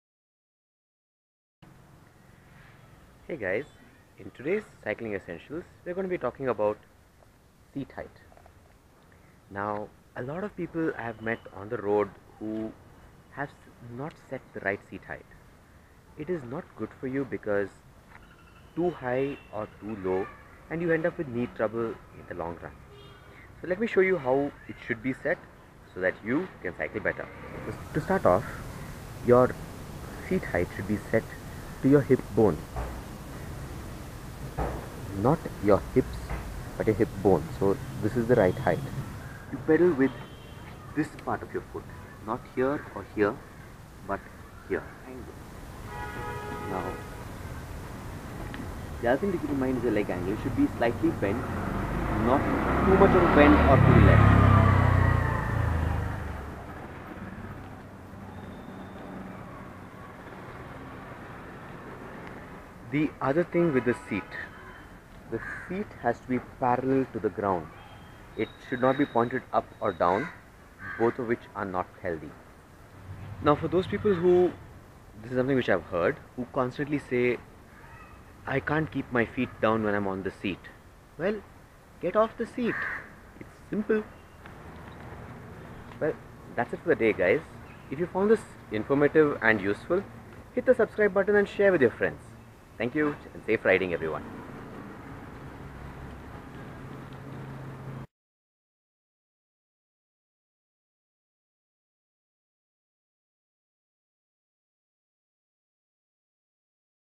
Seat Height Adjustment | How to | Cycling essentials | Ajsvlog | Indian Cycling Vlog
One of the most common mistakes newbie riders make is not setting the proper seat height. This mistake can prove costly in the long run with adverse effects on the knees. This is my small effort to help cyclists keep the proper seat height.

Music courtesy - Youtube music library

Camera - SJ4000 and SJ4000 wifi